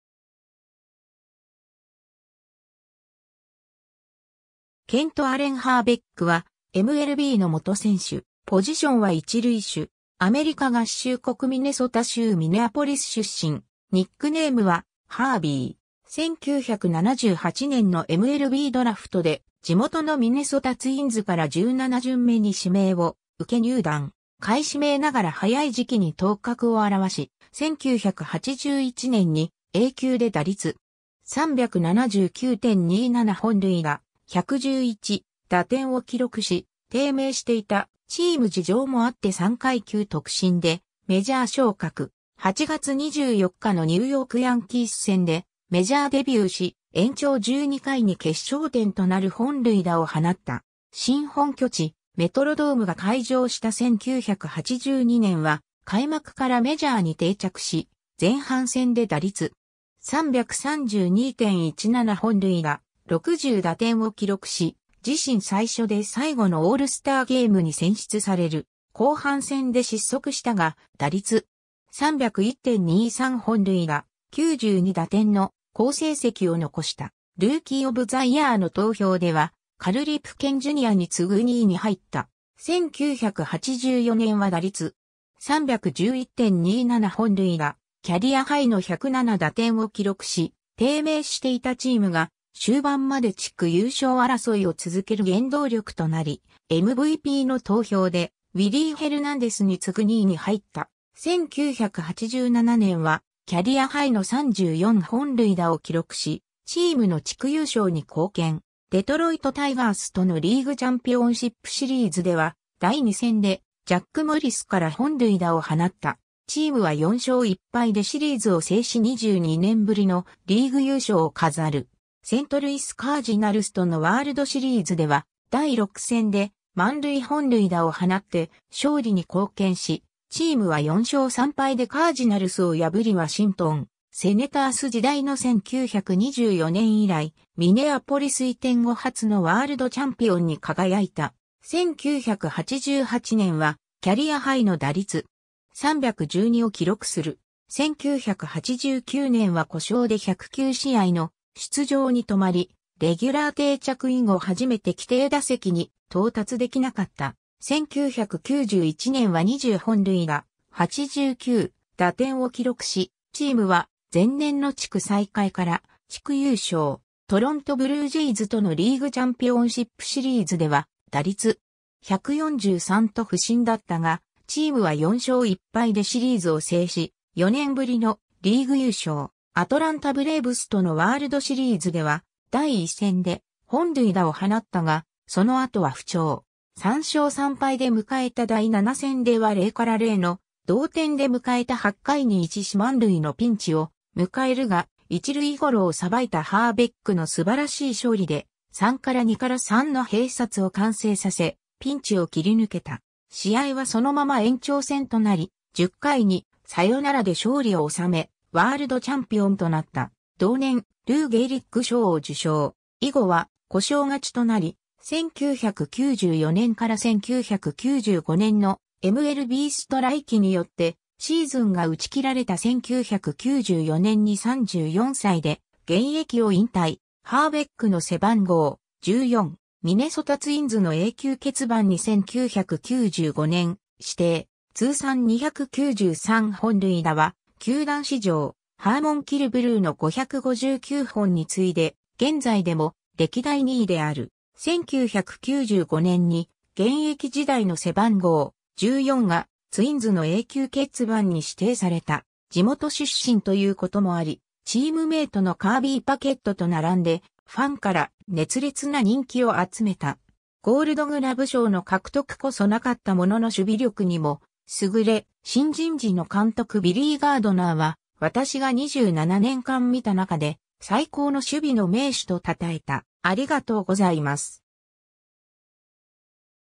ケント・ハーベック
ケント・アレン・ハーベック（Kent Allen Hrbek, 1960年5月21日 - ）は、MLBの元選手。
ポジションは一塁手。
アメリカ合衆国ミネソタ州ミネアポリス出身。
ニックネームは「Herbie」。
 1978年のMLBドラフトで地元のミネソタ・ツインズから17巡目に指名を受け入団。
下位指名ながら早い時期に頭角を現し、1981年にA級で打率.379・27本塁打・111打点を記録し、低迷していたチーム事情もあって3階級特進でメジャー昇格。
8月24日のニューヨーク・ヤンキース戦でメジャーデビューし、延長12回に決勝点となる本塁打を放った。
 新本拠地メトロドームが開場した1982年は開幕からメジャーに定着し、前半戦で打率.332・17本塁打・60打点を記録し、自身最初で最後のオールスターゲームに選出される。
後半戦で失速したが、打率.301・23本塁打・92打点の好成績を残した。
ルーキー・オブ・ザ・イヤーの投票ではカル・リプケン・ジュニアに次ぐ2位に入った。
1984年は打率.311・27本塁打、キャリアハイの107打点を記録し、低迷していたチームが終盤まで地区優勝争いを続ける原動力となり、MVPの投票でウィリー・ヘルナンデスに次ぐ2位に入った。
 1987年はキャリアハイの34本塁打を記録し、チームの地区優勝に貢献。
デトロイト・タイガースとのリーグチャンピオンシップシリーズでは第2戦でジャック・モリスから本塁打を放った。
チームは4勝1敗でシリーズを制し22年ぶりのリーグ優勝を飾る。
セントルイス・カージナルスとのワールドシリーズでは第6戦で満塁本塁打を放って勝利に貢献し、チームは4勝3敗でカージナルスを破りワシントン・セネタース時代の1924年以来、ミネアポリス移転後初のワールドチャンピオンに輝いた。
 1988年はキャリアハイの打率.312を記録する。
1989年は故障で109試合の出場に留まり、レギュラー定着以後初めて規定打席に到達できなかった。
 1991年は20本塁打・89打点を記録し、チームは前年の地区最下位から地区優勝。
トロント・ブルージェイズとのリーグチャンピオンシップシリーズでは打率.143と不振だったが、チームは4勝1敗でシリーズを制し、4年ぶりのリーグ優勝。
アトランタ・ブレーブスとのワールドシリーズでは第1戦で本塁打を放ったが、その後は不調。
3勝3敗で迎えた第7戦では0-0の同点で迎えた8回に1死満塁のピンチを迎えるが、一塁ゴロを捌いたハーベックの素晴らしい処理で3-2-3の併殺を完成させ、ピンチを切り抜けた。
試合はそのまま延長戦となり、10回にサヨナラで勝利を収め、ワールドチャンピオンとなった。
同年ルー・ゲーリッグ賞を受賞。
以後は故障がちとなり、1994年から1995年のMLBストライキによってシーズンが打ち切られた1994年に34歳で現役を引退。
 ハーベックの背番号「14」。
 ミネソタ・ツインズの永久欠番に1995年指定。
 通算293本塁打は、球団史上ハーモン・キルブルーの559本に次いで現在でも歴代2位である。
1995年に現役時代の背番号『14』がツインズの永久欠番に指定された。
 地元出身ということもあり、チームメイトのカービー・パケットと並んでファンから熱烈な人気を集めた。
ゴールドグラブ賞の獲得こそなかったものの守備力にも優れ、新人時の監督ビリー・ガードナーは「私が27年間見た中で（一塁手としては）最高の守備の名手」と称えた。